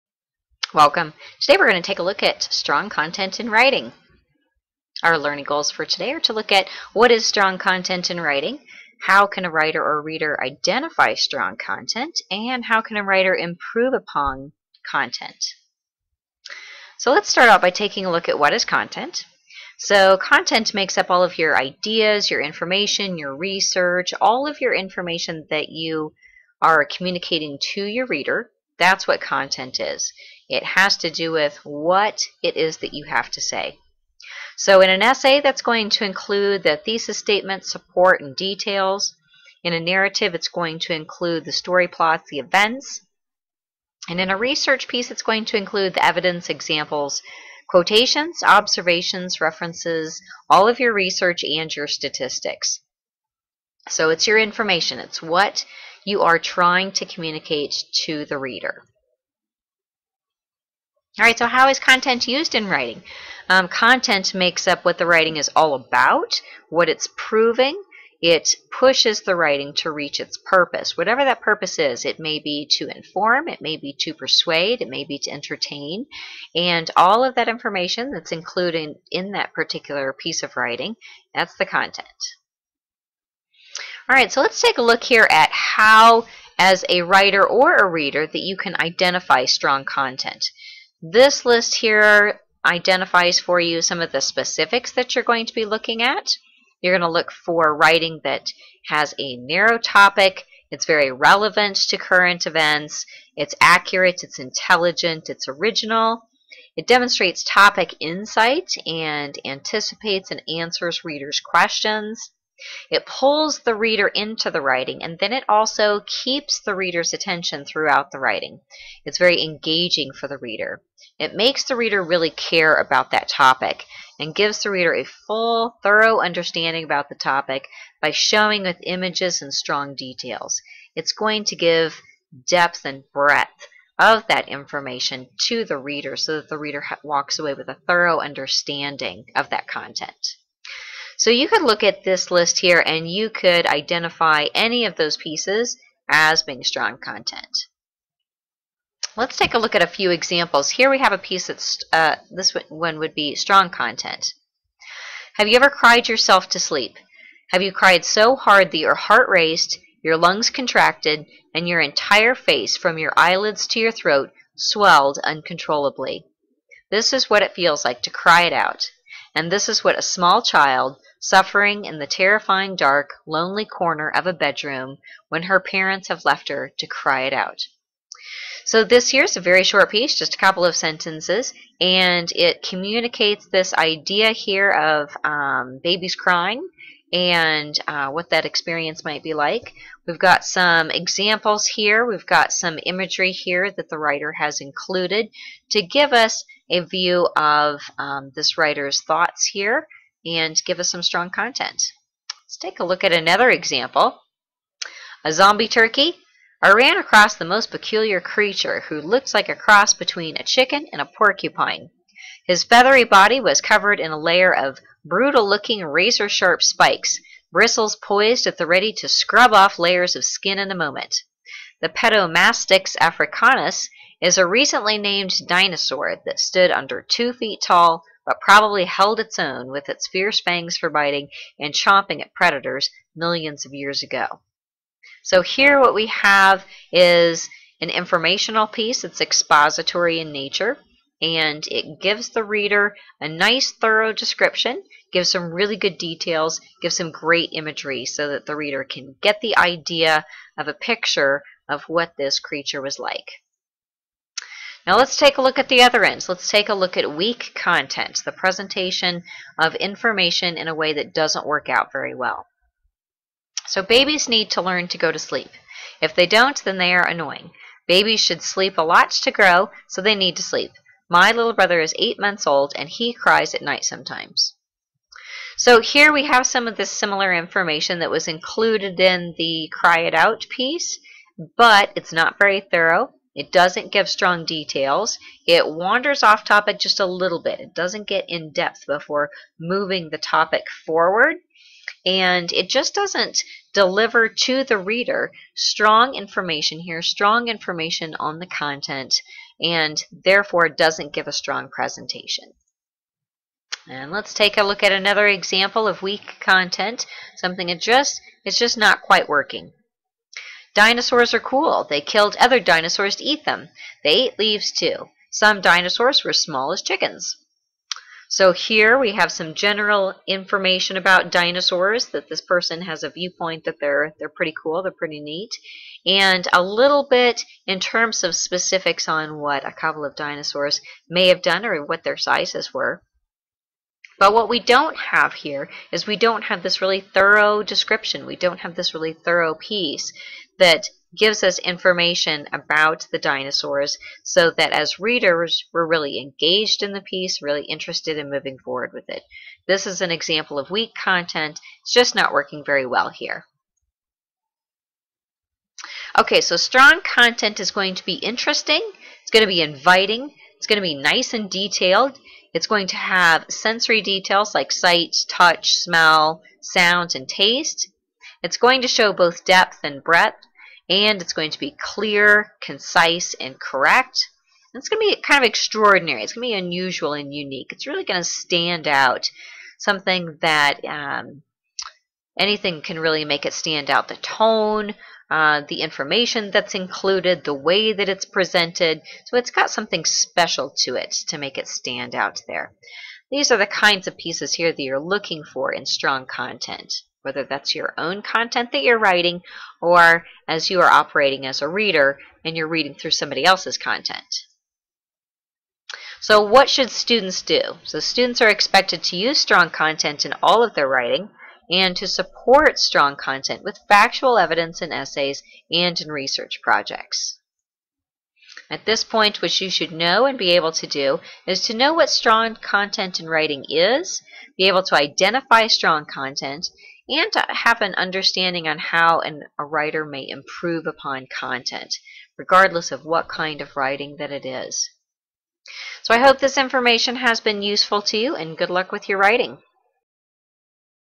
Become a Better Writer: Strong Ideas and Content in Writing
Want to be a better writer?  for book recommendations, writing tips, and pictures of my typewriters.